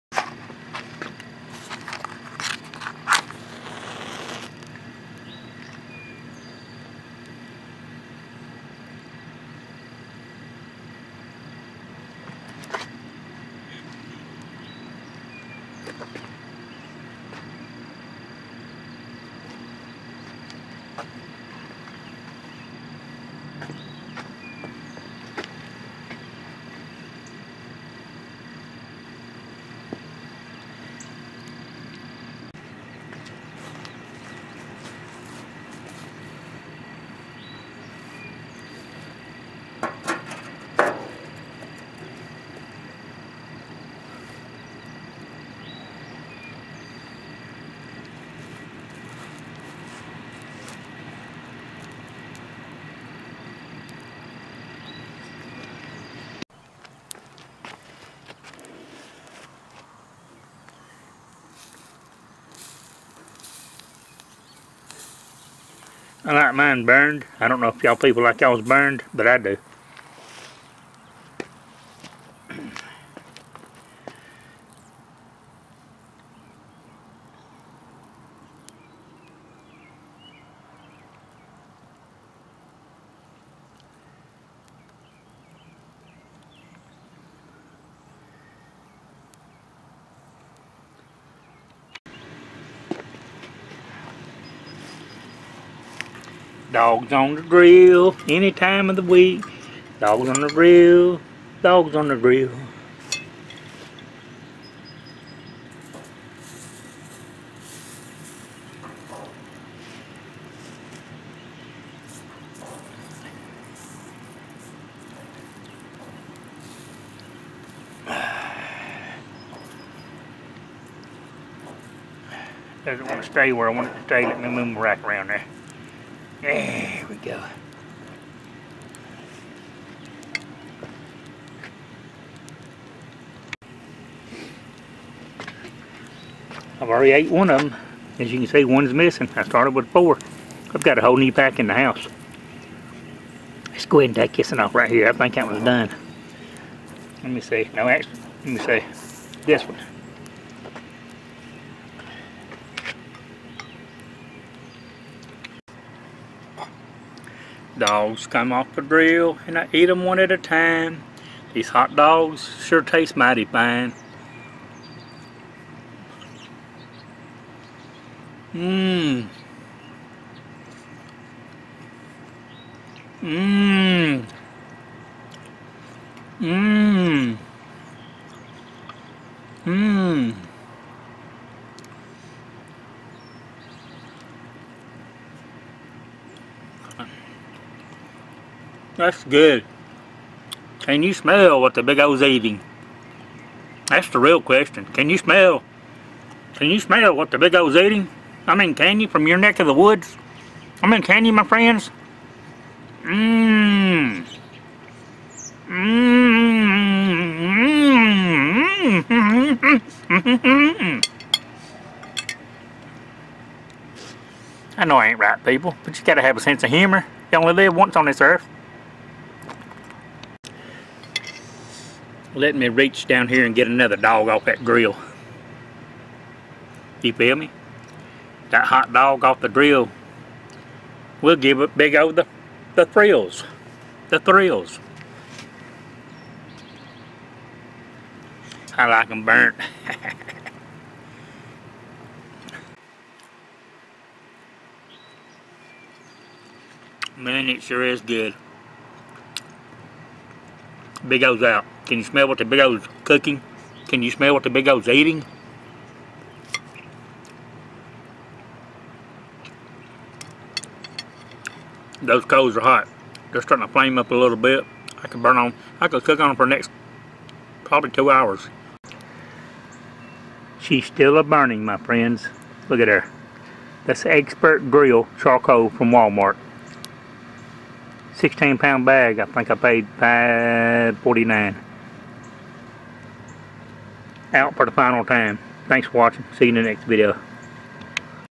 HOW TO COOK HOT DOGS ON A GRILL | FIRE PIT
Join me as I cook delicious hot dogs on my iron fire pit OUTSIDE! I like to hear from my viewers, my friends. Have a wonderful day. Be happy, positive, and focused. If you ain't laughing, you ain't living!